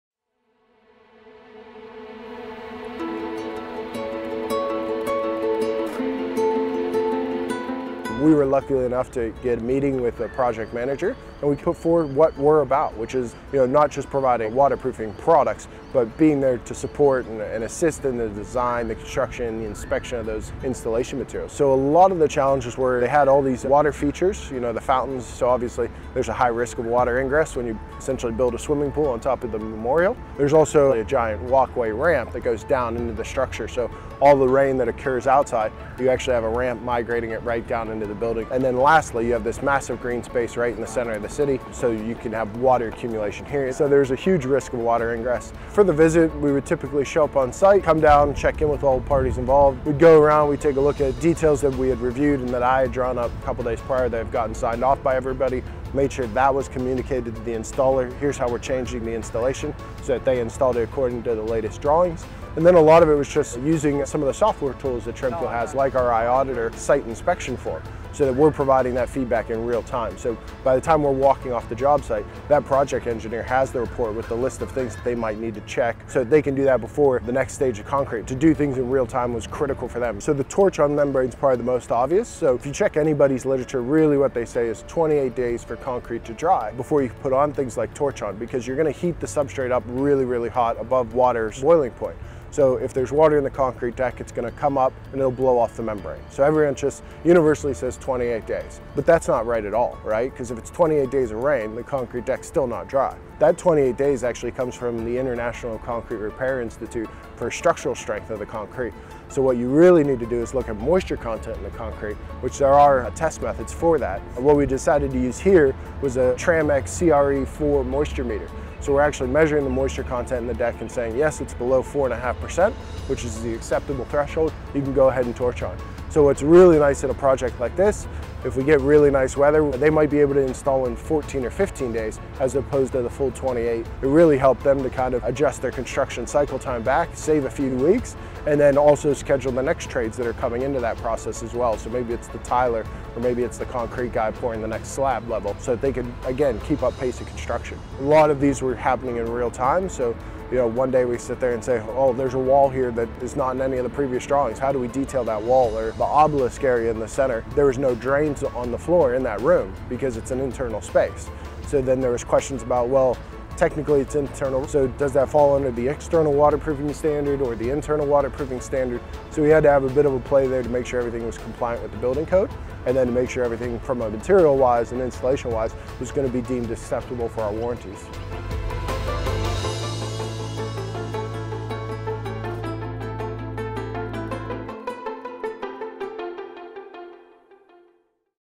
Challenges of Waterproofing Anzac
When restoring a National War Monument visited by hundreds of thousands of people, you need to provide the highest quality results and finishes expected of such a significant structure in our history. Tremco worked closely with Sure Seal and Built to provide a solution which was both timely and high quality.